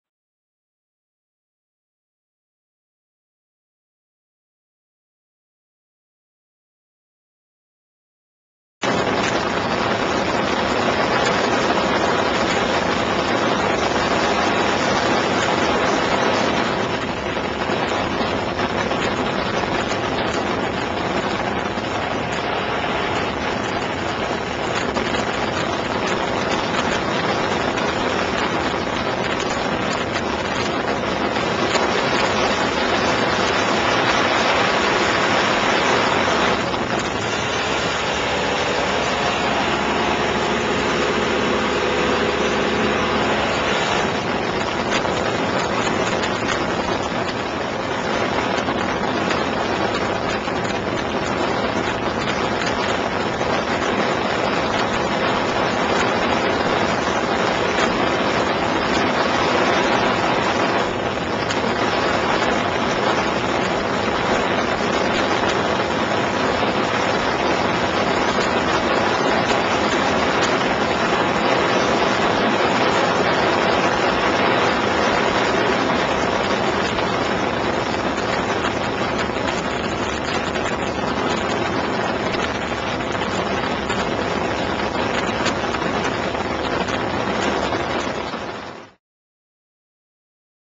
Mt. Index Flyby
Super cub floatplane flyby of Mt. Index